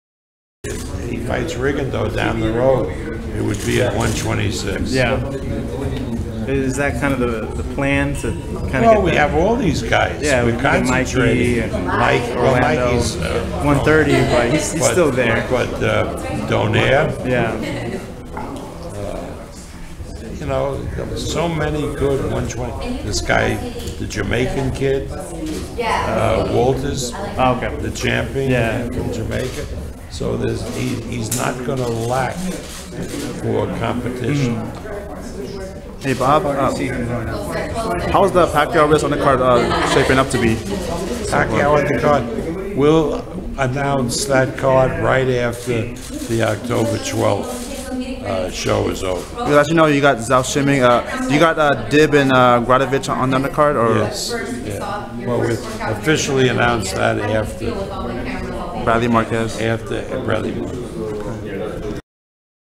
Bob Arum on Vasyl Lomachenko's future opponents and status on the Pacquiao-Rios undercard
Bob Arum on Vasyl Lomachenko and update on the Pacquiao-Rios undercard.
Youtube.com/SixerIverson04
Youtube.com/ChrisDeGuia